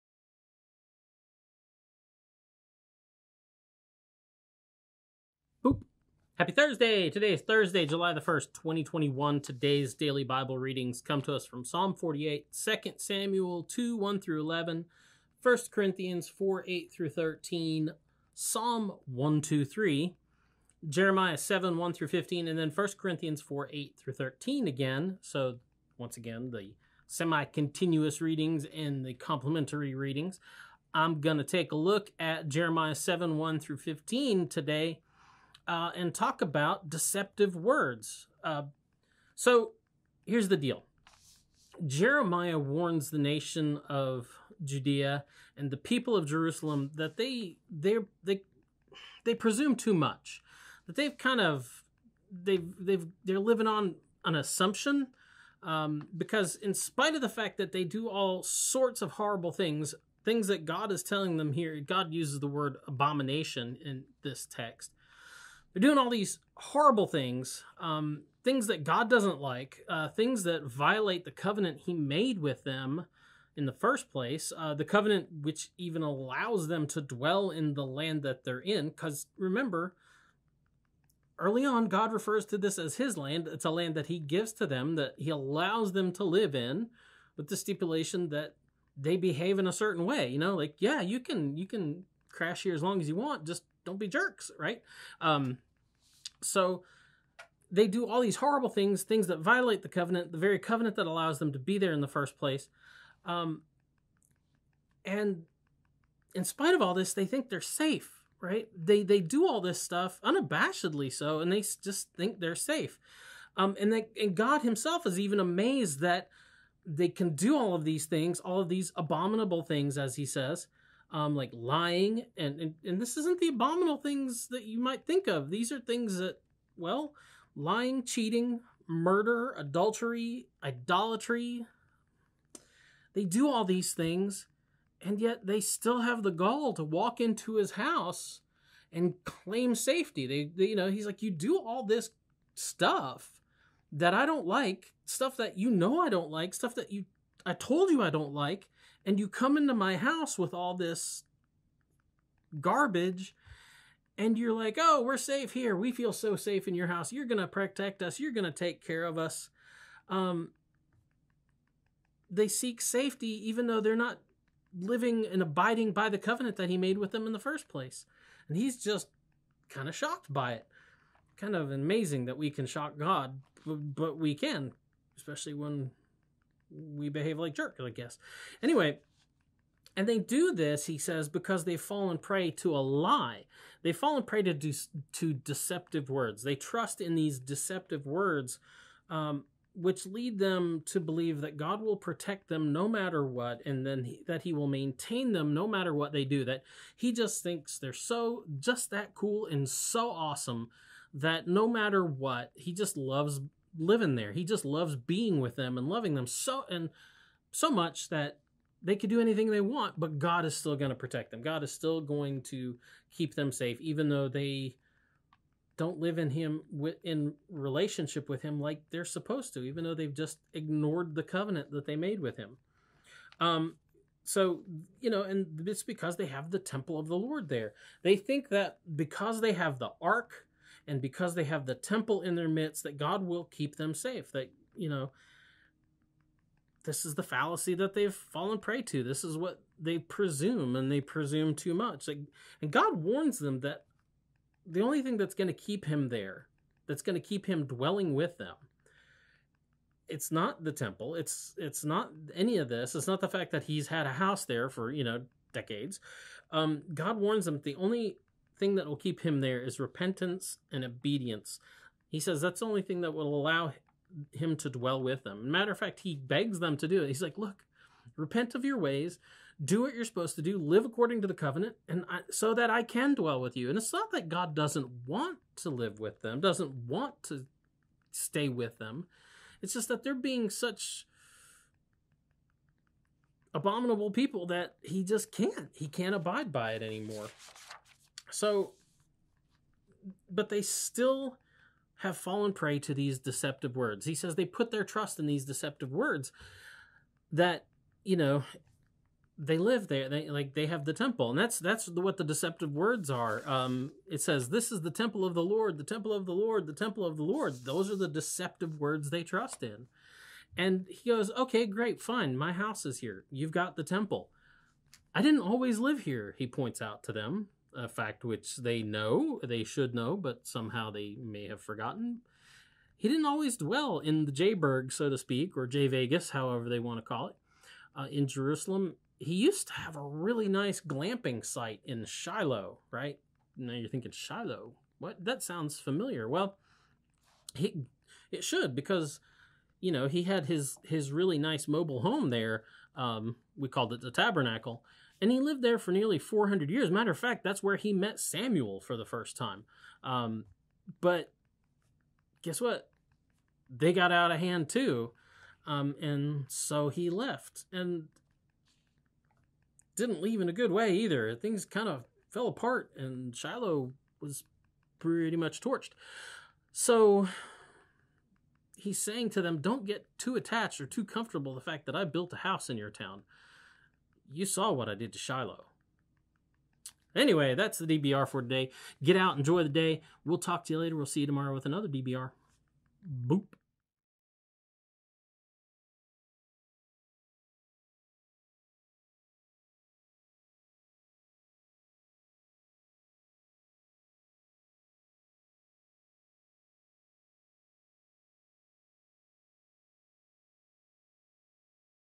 Daily Bible Reading for July 1, 2021 "Deceptive Words"
Today in Wesley's on YouTube, God reminds people what he did to Shiloh. "Deceptive Words" Jeremiah 7:1-15

Daily Bible Readings for Thursday, July 1, 2021:
Semi-continuous: Psalm 48; 2 Samuel 2:1-11; 1 Corinthians 4:8-13
Complementary: Psalm 123; Jeremiah 7:1-15; 1 Corinthians 4:8-13